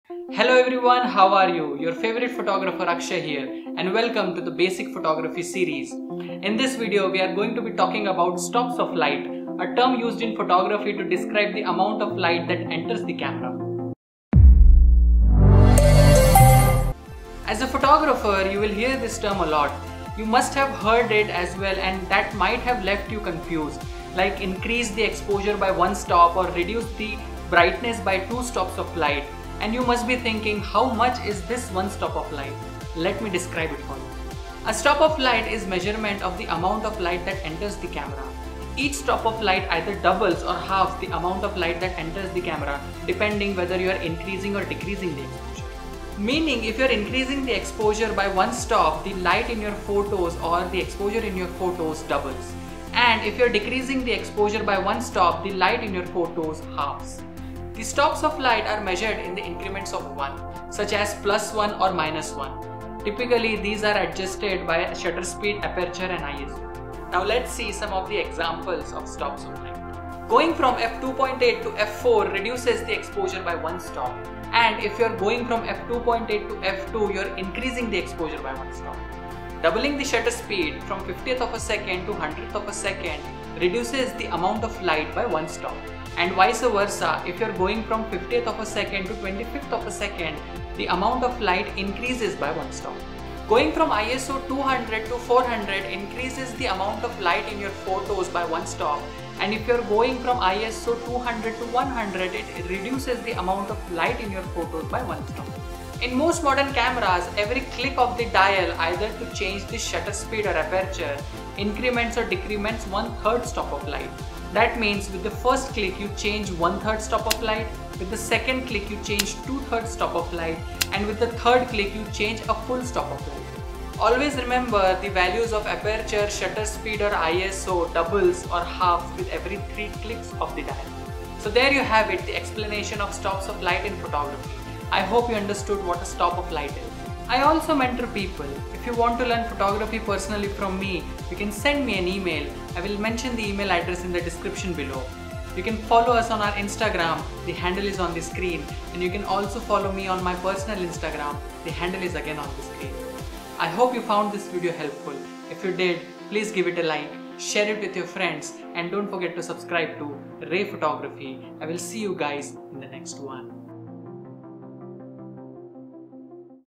Know What Stops of Light Are | Stops of Light in Photography
In this video, I have explained what stops of light are in photography, and how you can use them to take better control over the exposure of your photos.